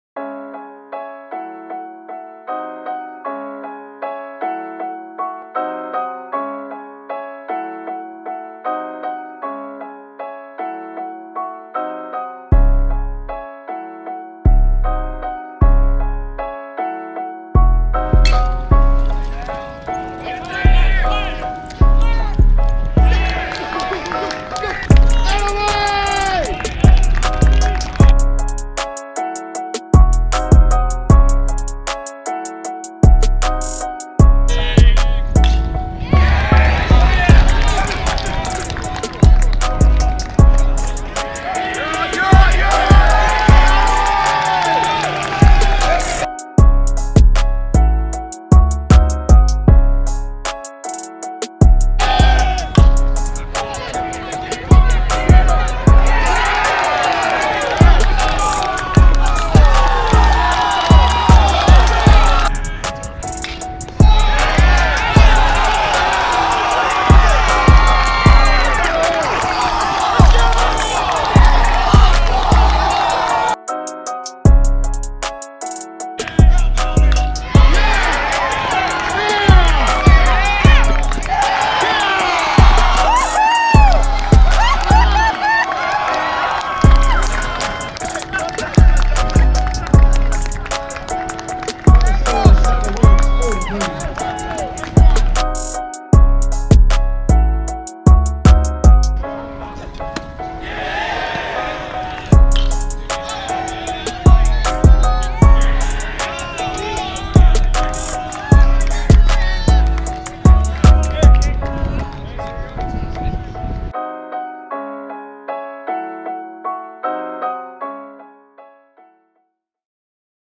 Baseball Highlights: UChicago Beats Monmouth Twice in Doubleheader (April 3, 2022)
Video shot and edited by Max Brandstetter.
Baseball game highlights from University of Chicago's home wins over Monmouth College on April 3, 2022.